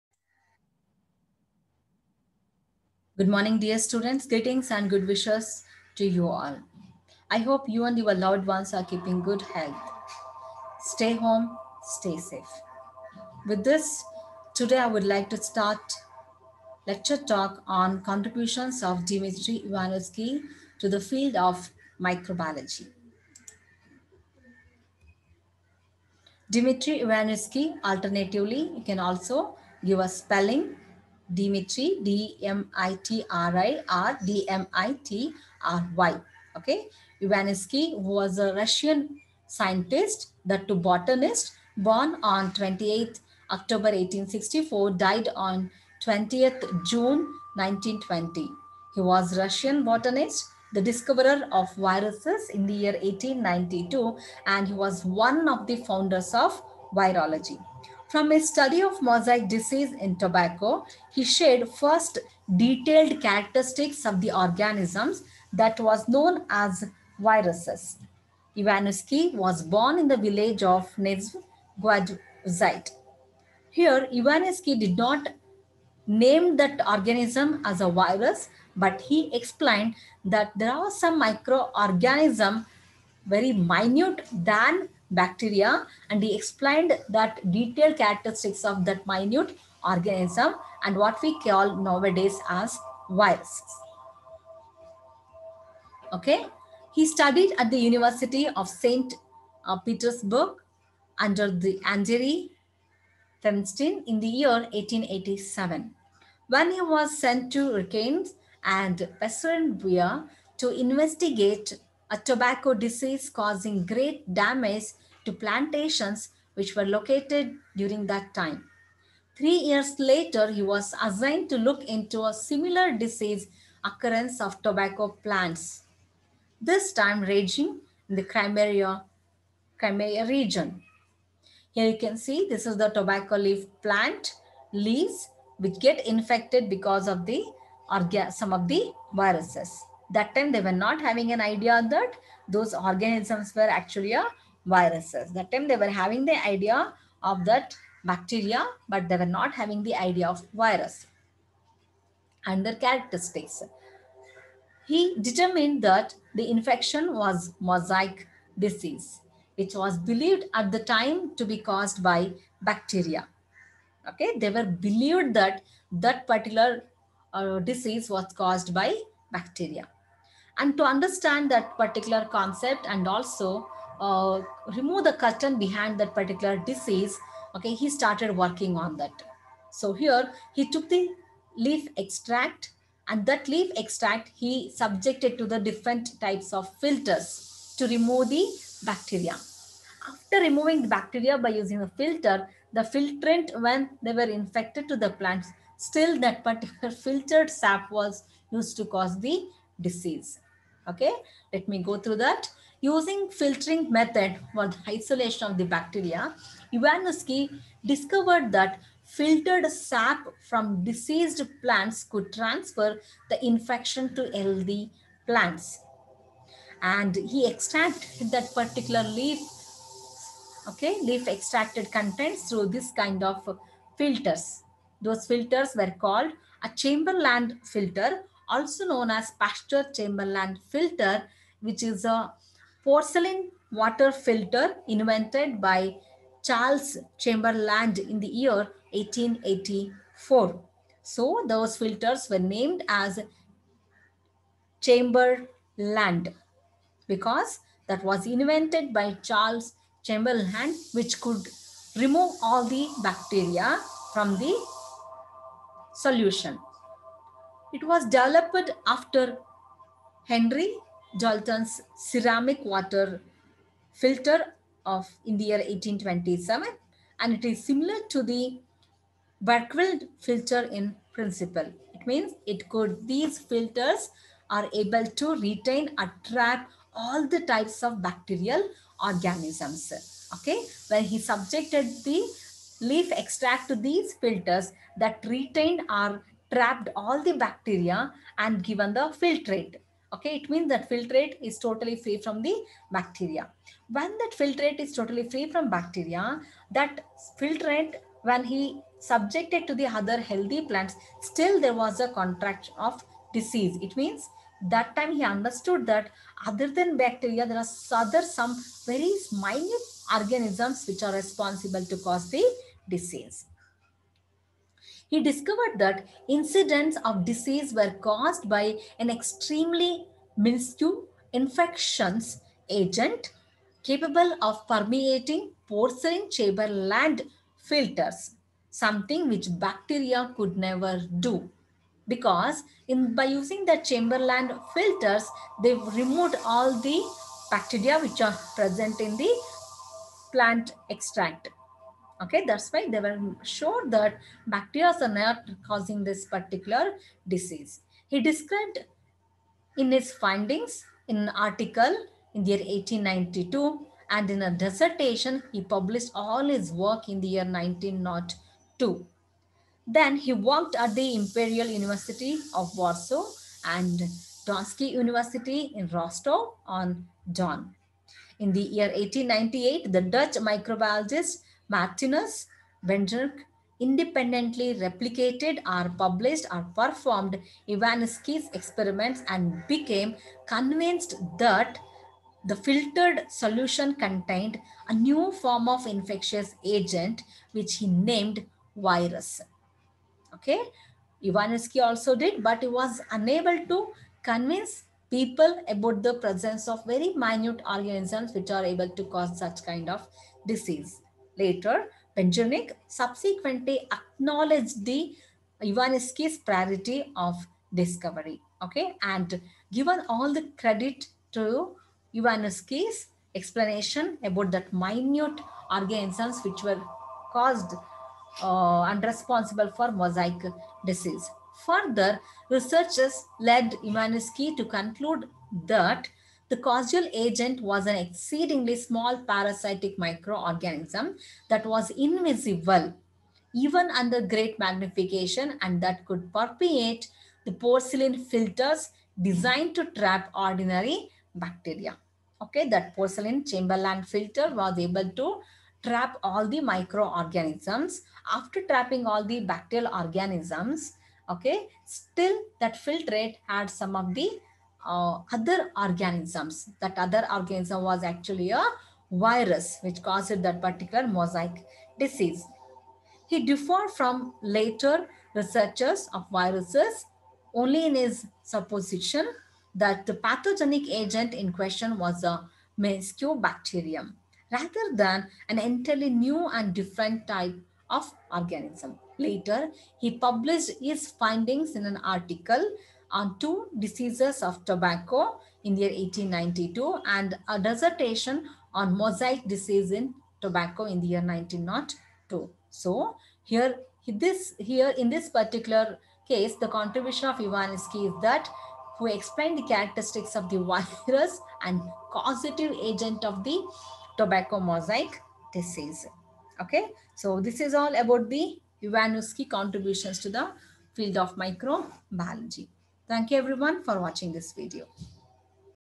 Contributions of Dmitri Iwanovski to the field of Microbiology
Dmitri Ivanovsky (alternative spelling Dmitrii or Dmitry Iwanowski, Russian  born on  28 October 1864 –  died on 20 June 1920) was a Russian botanist, the discoverer of viruses (1892) and one of the founders of virology
From his study of mosaic disease in tobacco, shared first detailed characteristics of the organisms that was  known as viruses.